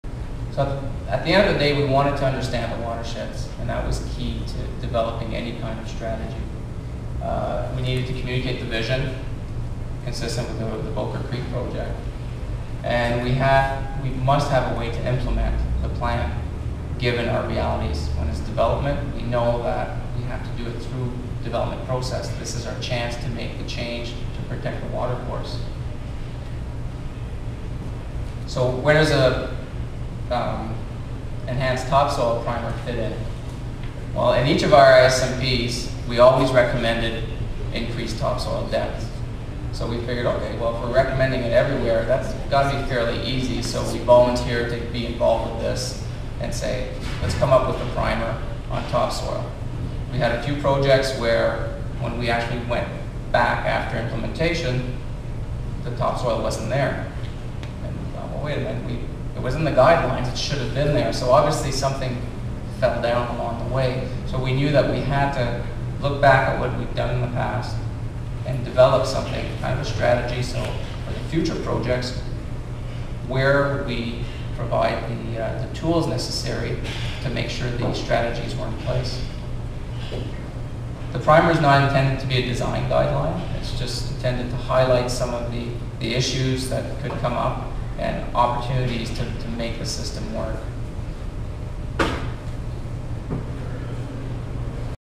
Need for Topsoil Primer Set
“The development process is our opportunity to make the change to protect our watercourses. In each of our watershed plans, we always recommend increased topsoil depth. So the City volunteered to develop a primer for implementation," stated Remi Dube.

On February 23, 2010, the Bowker Creek Forum in Victoria celebrated urban watershed management successes in the Georgia Basin, and was the venue for the formal release of the Topsoil Law and Policy and Technical Primer Set.